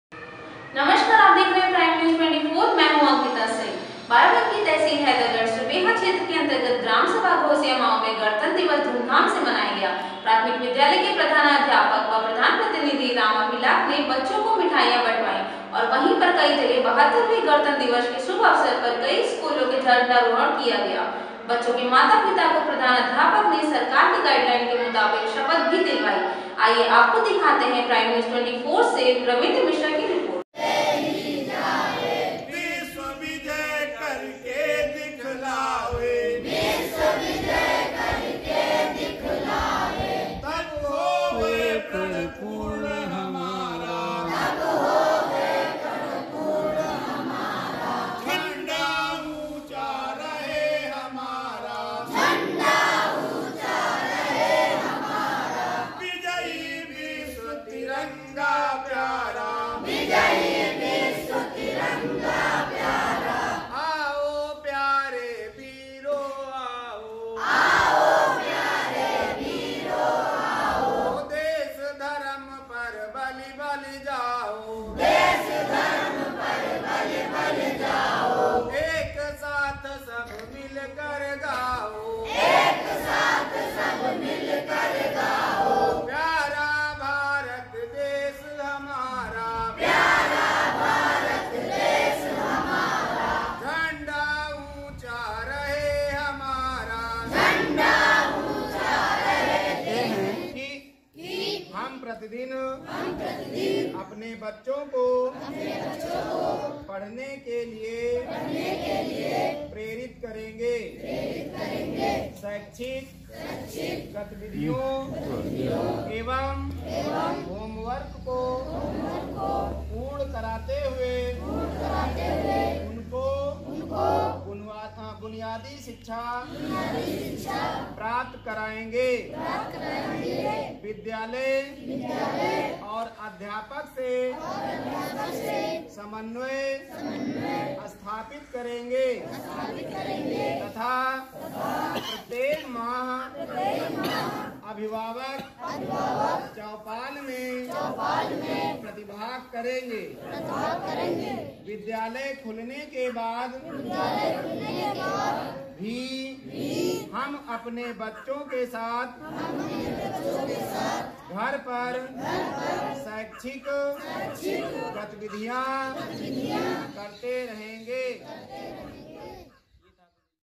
ग्राम सभा घोसिया मऊ में गणतंत्र दिवस धूमधाम से मनाया गया
बाराबंकी तहसील हैदरगढ़ सुबेहा क्षेत्र के अंतर्गत ग्राम सभा घोसिया मऊ में गणतंत्र दिवस धूमधाम से मनाया गया.प्राथमिक विद्यालय के प्रधानाध्यापक व प्रधान प्रतिनिधि राम अभिलाख ने बच्चों को मिठाइयां बटवा या और वहीं पर कई जगह 72वेंगणतंत्रदिवस के शुभ अवसर पर कई स्कूलों में झंडारोहण किया गया बच्चों के माता पिता को प्रधानाध्यापक ने सरकार के गाइडलाइन के मुताबिक शपथ भी दिलाई।